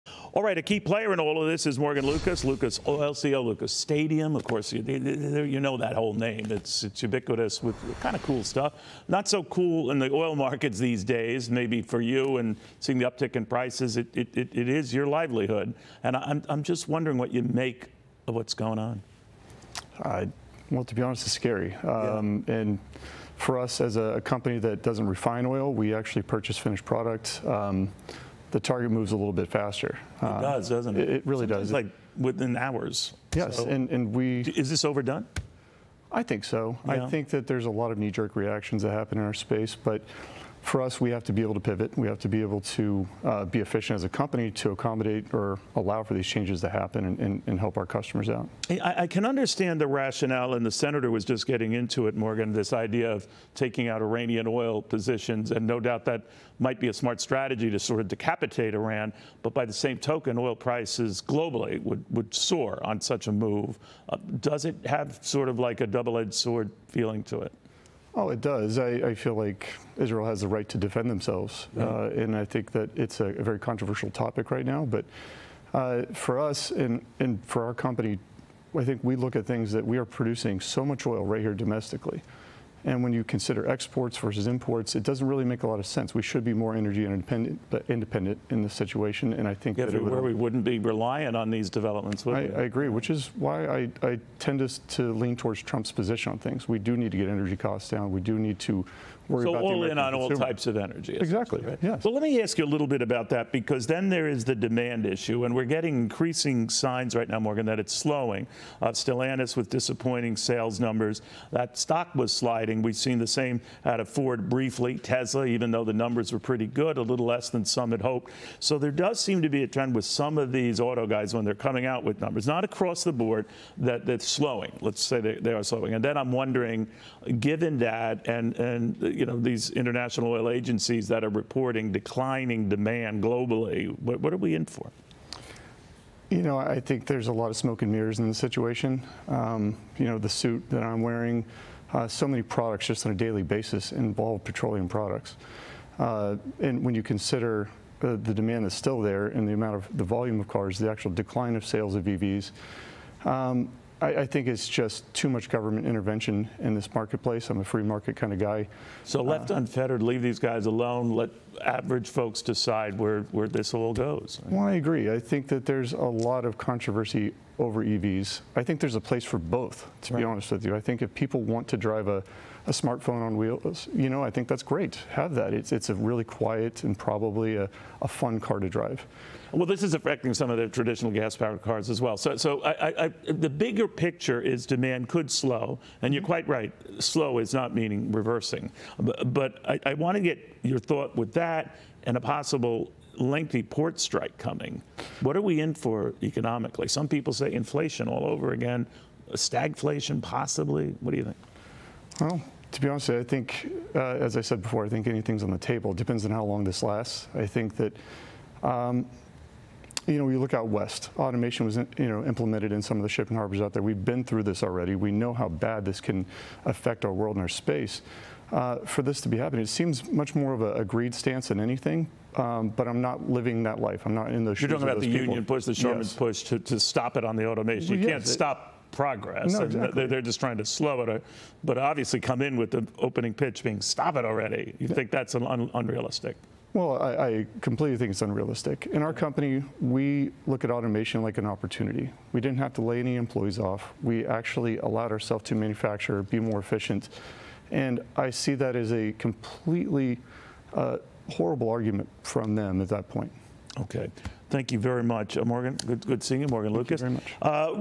Oil expert: There's too much government intervention in this marketplace
Lucas Oil CEO Morgan Lucas discusses the impact of the Middle East conflict on oil, the demand for crude oil and automation being at the helm of the port worker strike.